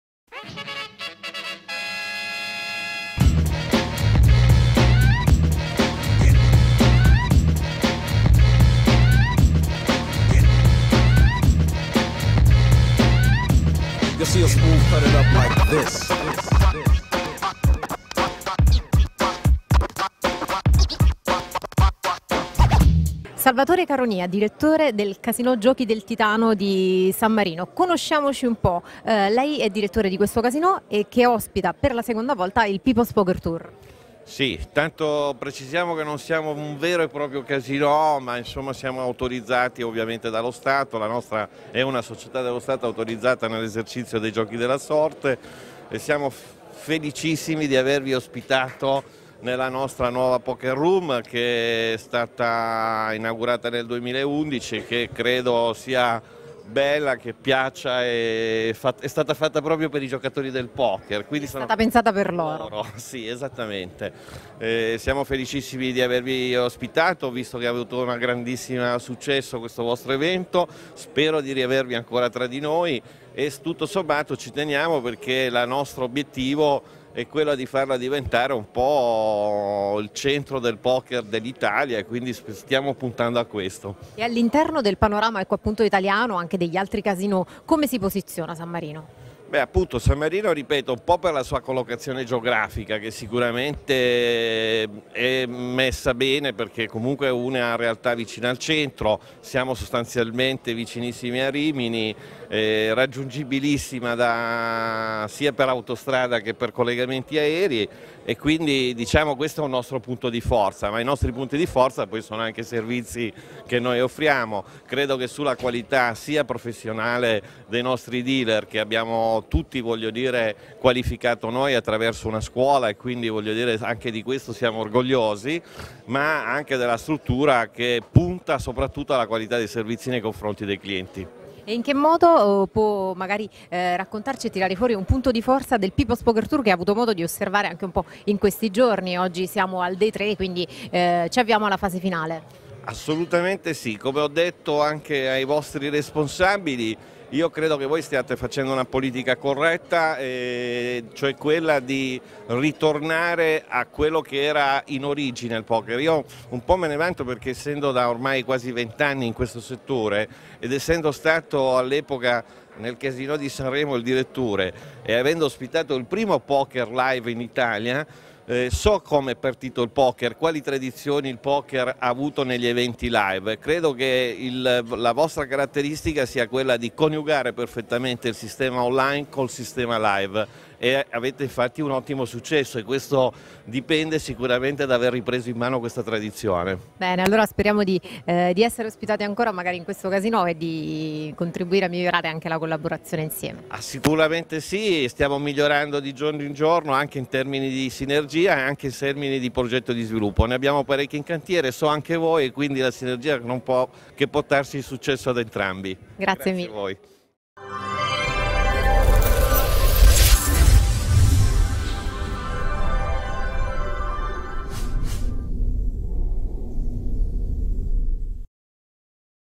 PPTour San Marino 2015 - Intervista a Salvatore Caronìa
Guarda l'intervista al direttore del Casinò Giochi del Titano, Salvatore Caronìa.